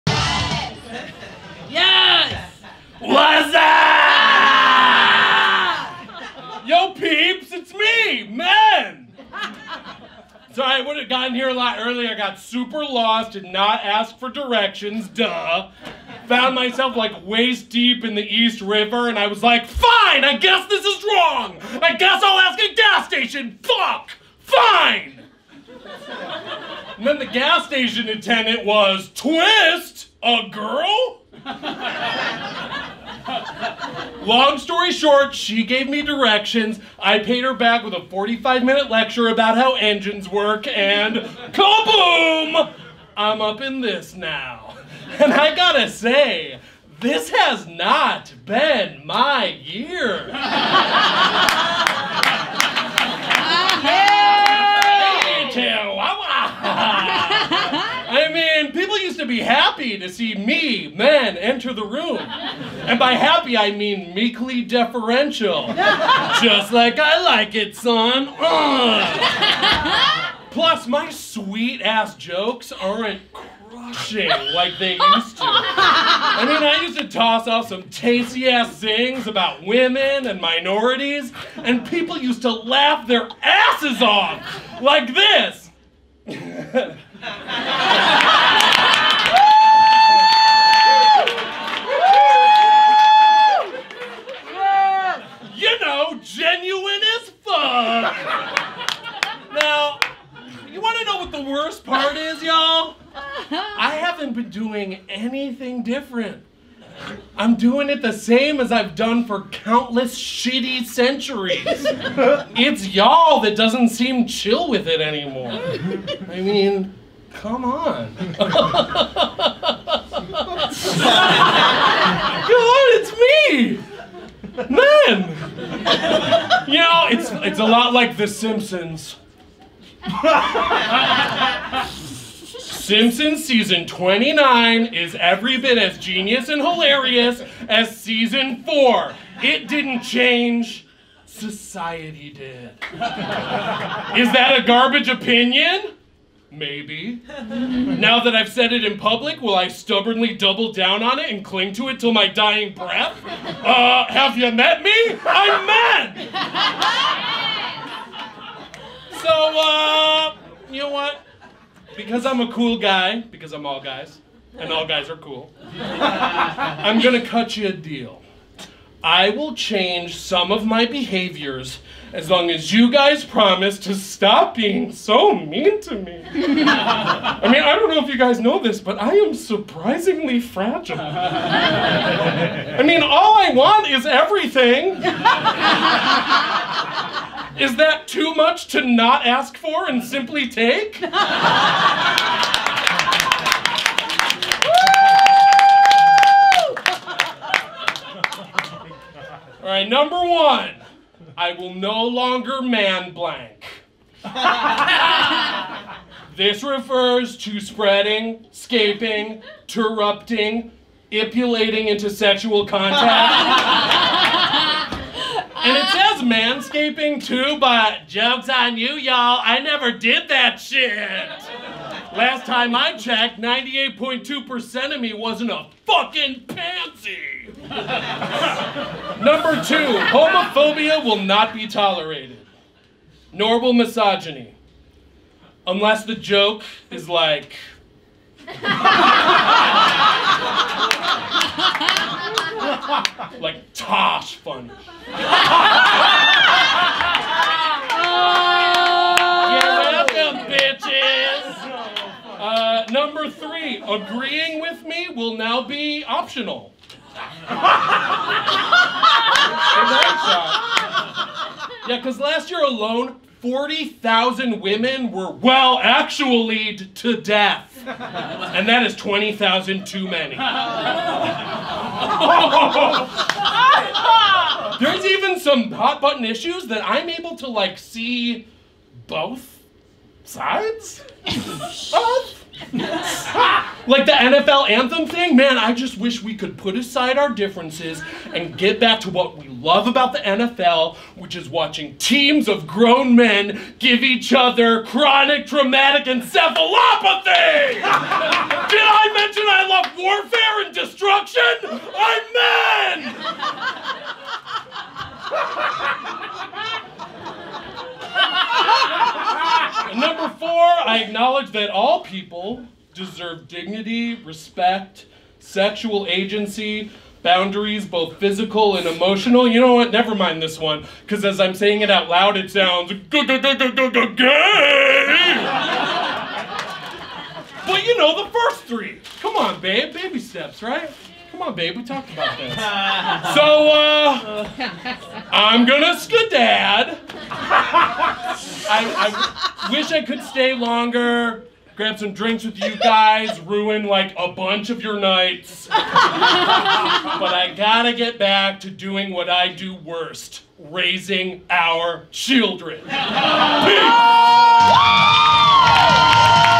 Men
Men will make some changes going forward if you all promise to stop being so mean!

Written and performed by Michael Hartney